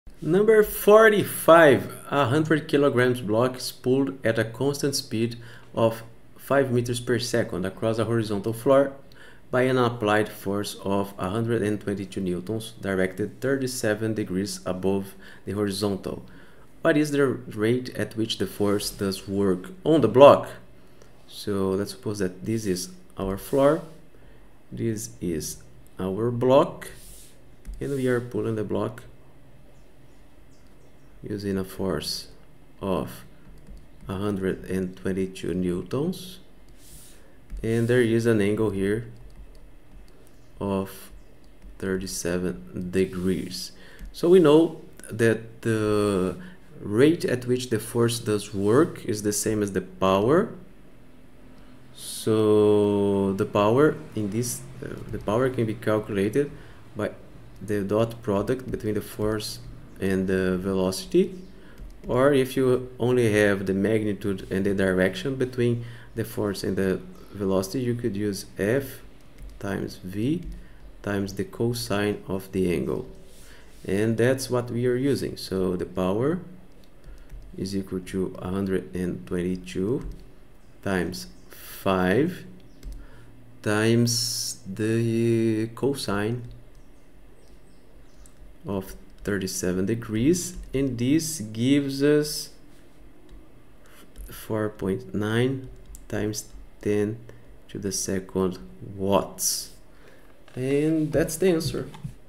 HALLIDAY SOLUTIONS - CHAPTER 7 PROBLEM 45 - Fundamentals of Physics 10th
A 100 kg block is pulled at a constant speed of 5.0 m/s across a horizontal floor by an applied force of 122 N directed 37 above the horizontal. What is the rate at which the force does work on the block?